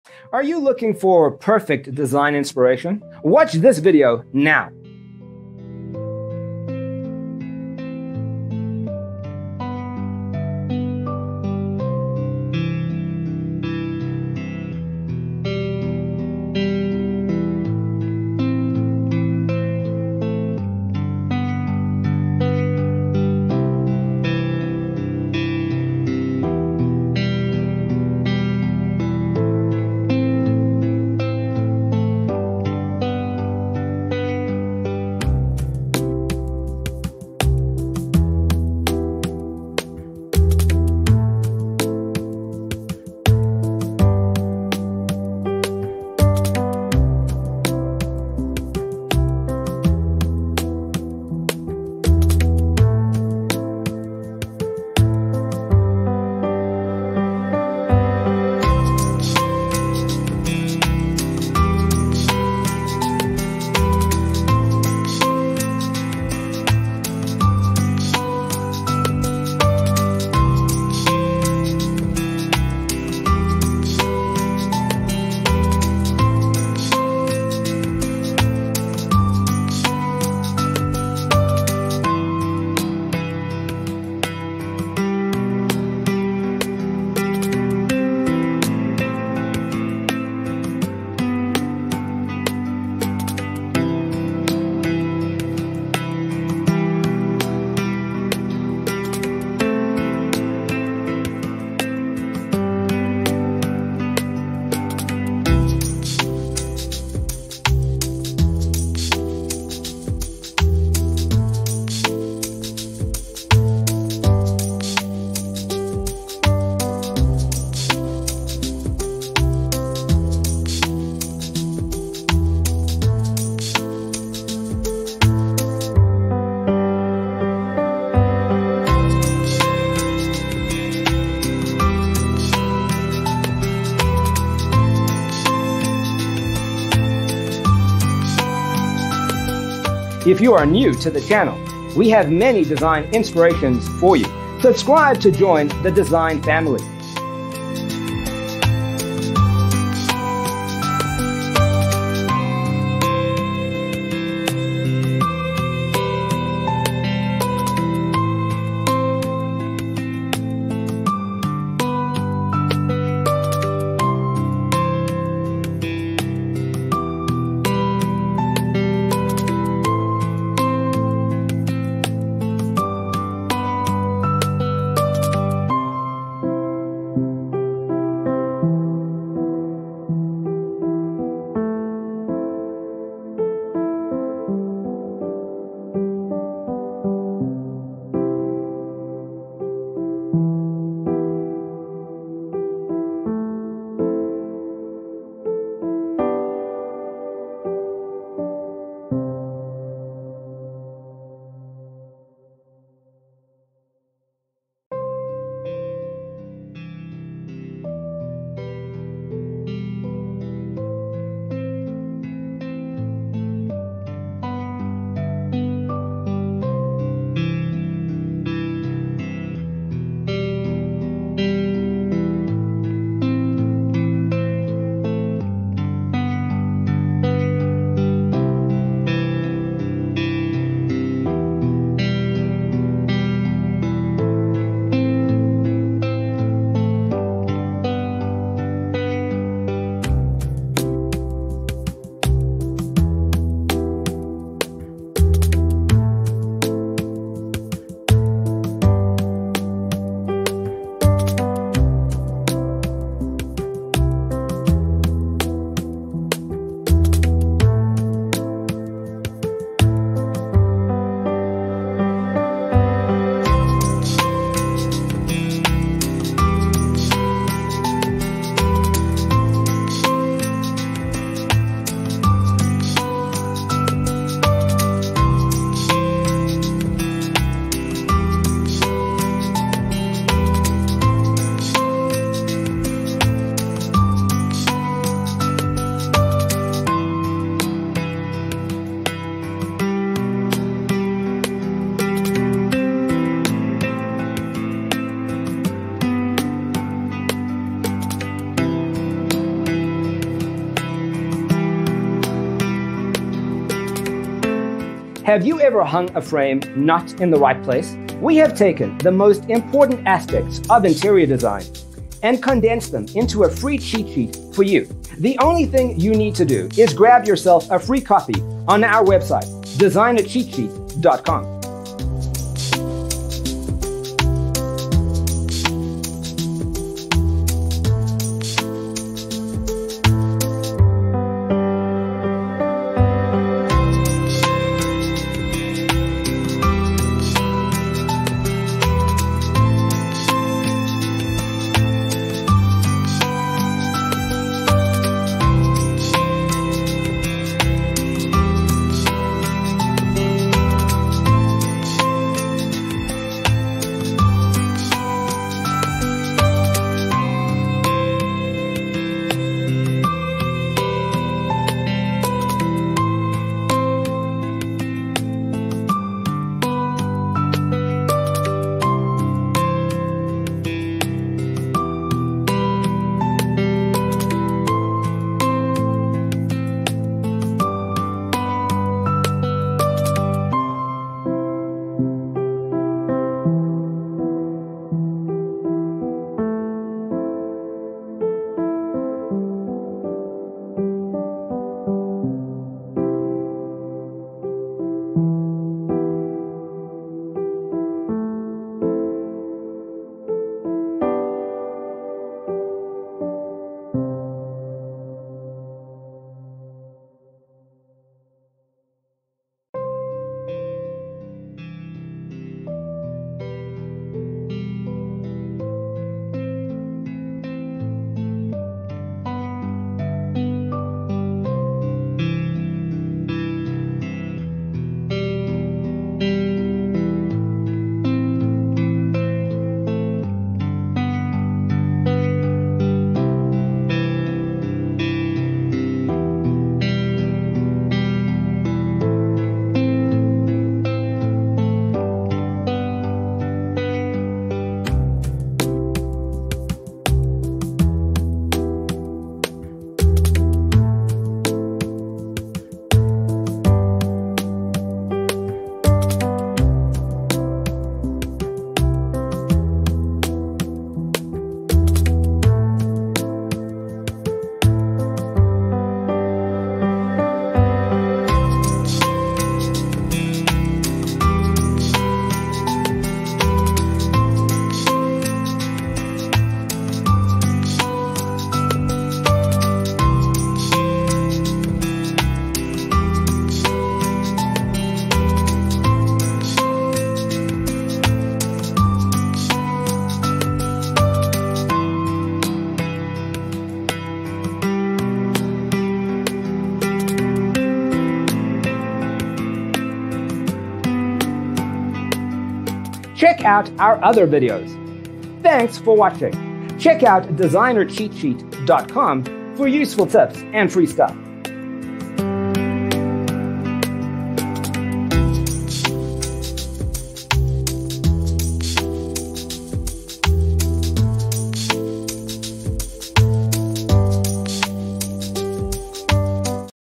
75 All Siding Materials French Country Exterior Home Design Ideas You'll Love ⭐️
Browse french country exterior home design photos. Discover decor ideas and architectural inspiration to enhance your homes french country exterior and facade as you build or remodel.

0:00:00 - Intro
0:00:12 - All Siding Materials French Country Exterior Home Design Ideas You'll Love: Inspirations 1 to 25
0:03:03 - All Siding Materials French Country Exterior Home Design Ideas You'll Love: Inspirations 26 to 50
0:06:07 - All Siding Materials French Country Exterior Home Design Ideas You'll Love: Inspirations 51 to 75